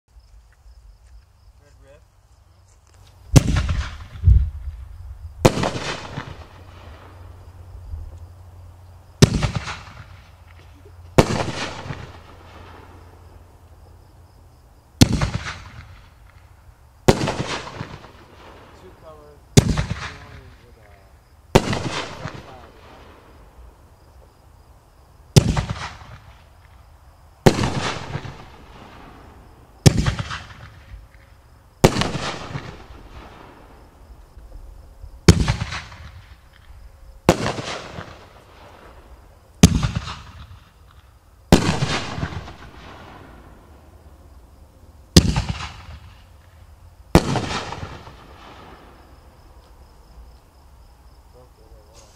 Red Riff - 9 Shot
A riff on the color RED - brocade ring with sea blue stars to RED stars and orange stars to RED stars. Brocade ring with lemon stars to purple stars and RED stars with blue stars. Brocade ring with purple and green stars and silver stars to RED stars. Ends with a 3-shot finale.

Approximate height reflects final camera position.